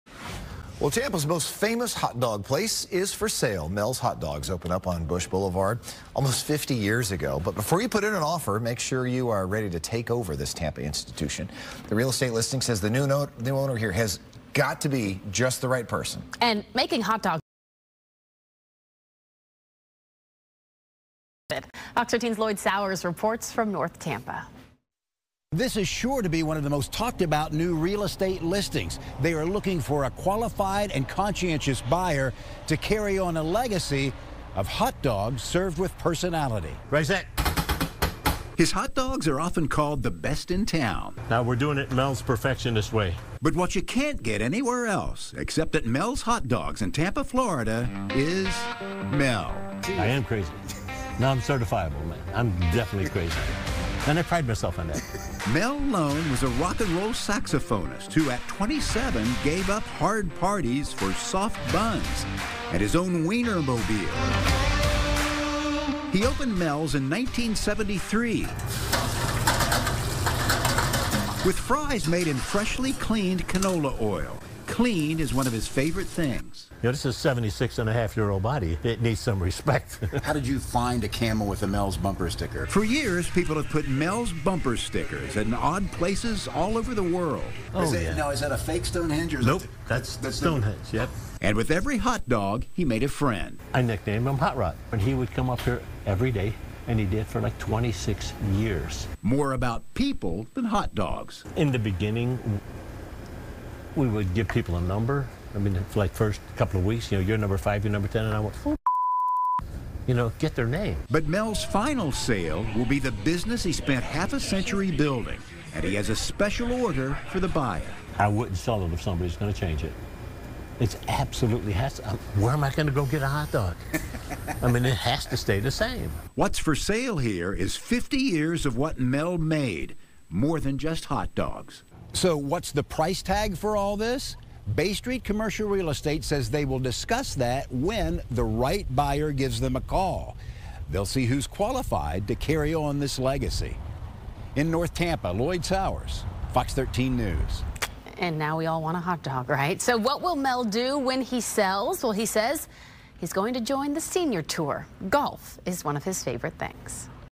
Famous Florida hotdog stand seeks new owner after 50 years
The iconic Mel’s Hot Dog restaurant in Tampa is up for sale to a "qualified and conscientious" buyer "to carry on a very special legacy."